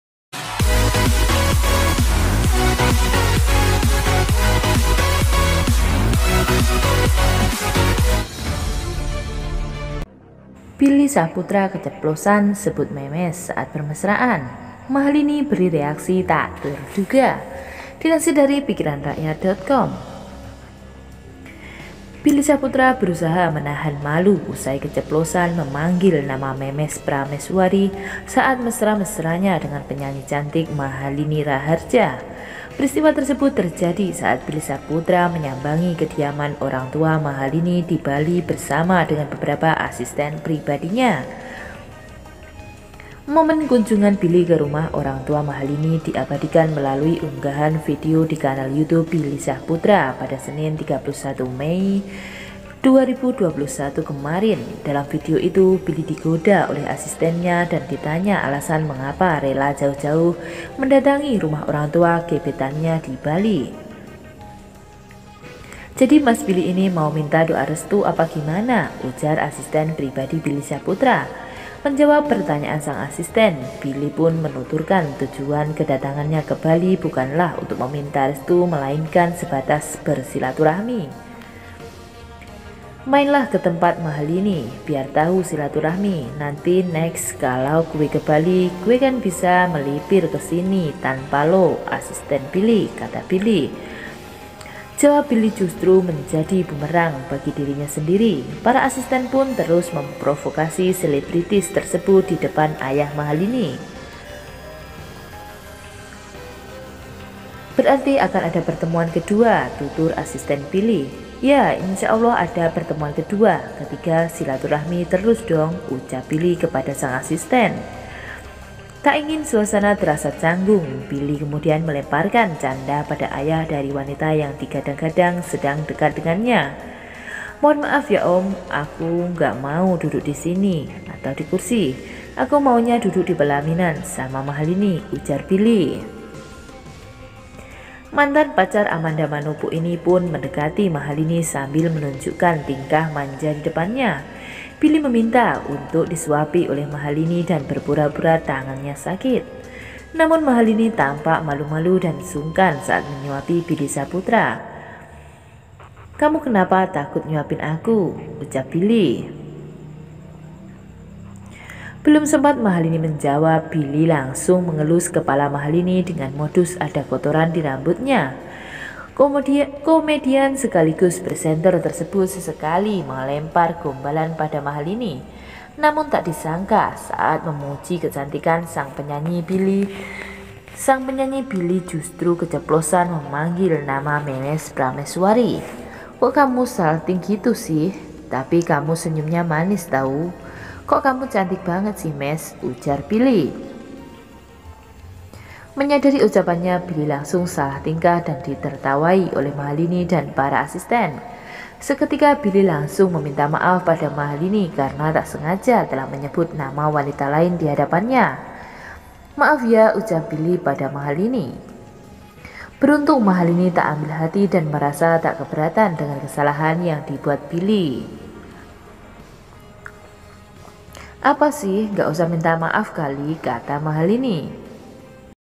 Billy Syahputra Keceplosan Sebut Memes Saat Bermesraan, Mahalini Beri Reaksi Tak Terduga
Billy Syahputra berusaha menahan malu usai keceplosan memanggil nama Memes Prameswari saat mesra-mesranya dengan penyanyi cantik Mahalini Raharja.

Peristiwa tersebut terjadi saat Billy Syahputra menyambangi kediaman orangtua Mahalini di Bali bersama dengan beberapa asisten pribadinya.

Momen kunjungan Billy ke rumah orangtua Mahalini diabadikan melalui unggahan video di kanal YouTube Billy Syahputra pada Senin, 31 Mei 2021 kemarin.